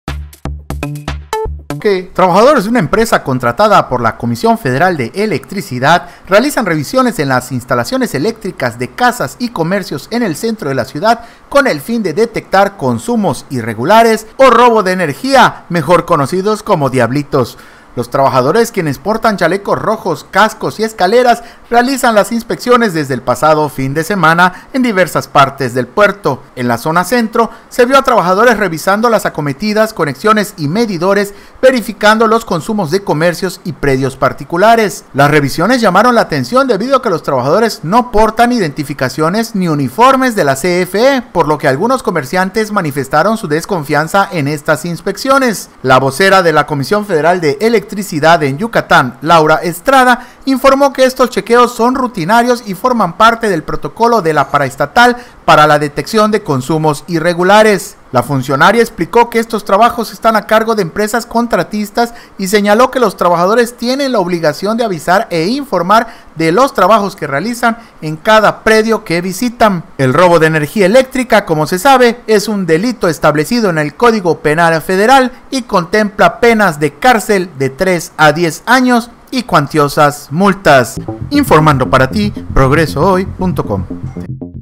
La CFE busca "diablitos" en casas y negocios de Progreso
Trabajadores de una empresa contratada por la Comisión Federal de Electricidad (CFE), realizan revisiones en las instalaciones eléctricas de casas y comercios en el Centro de la ciudad con el fin de detectar consumos irregulares o robo de energía eléctrica.